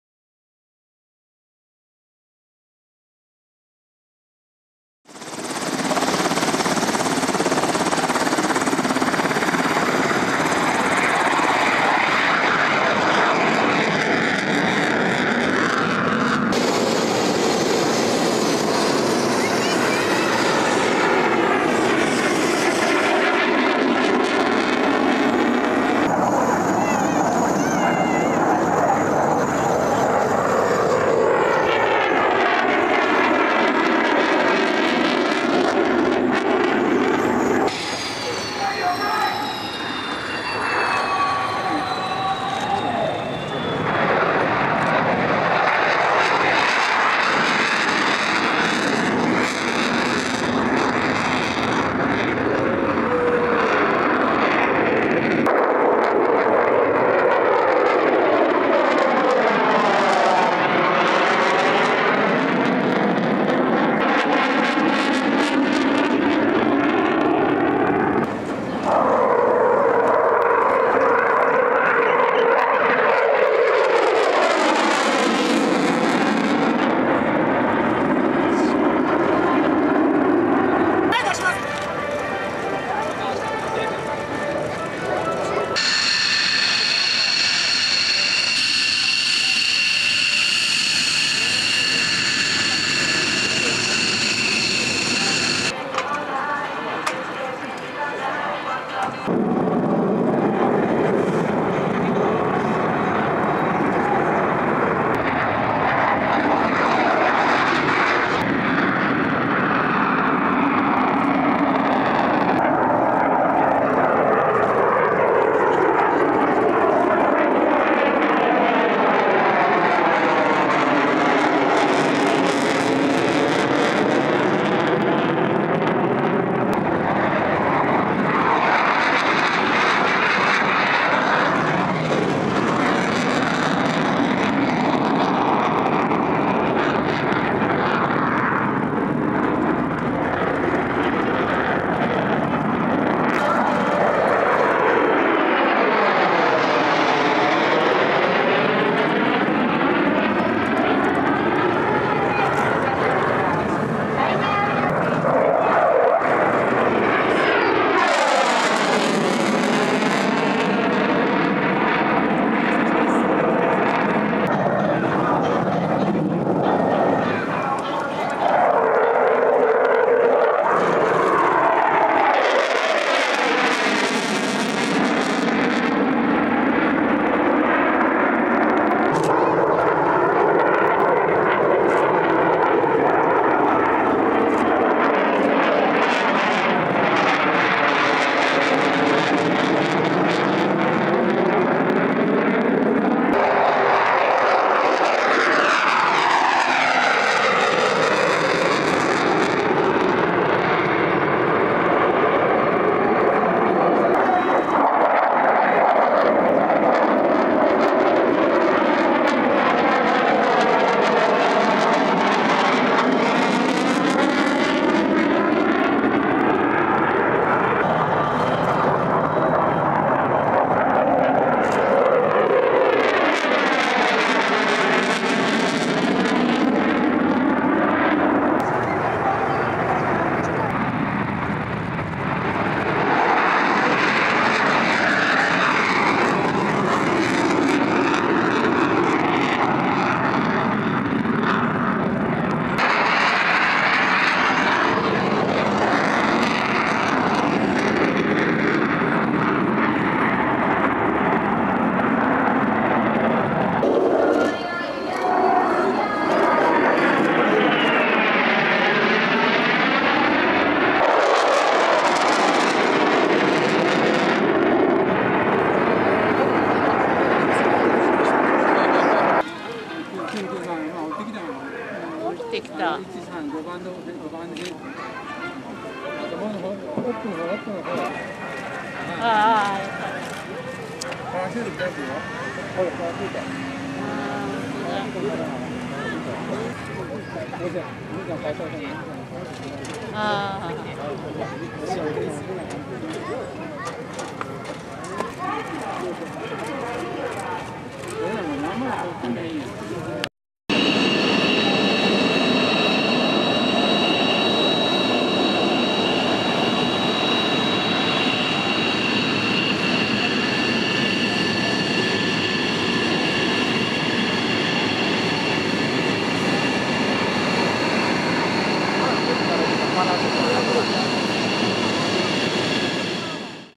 F4EJ改 2011岐阜基地 航空祭
2011岐阜基地　航空祭のF4EJ改の飛行をまとめました。
Pentax K-r
A17P AF 70-300mm F4-5.6 Di LD MACRO 1:2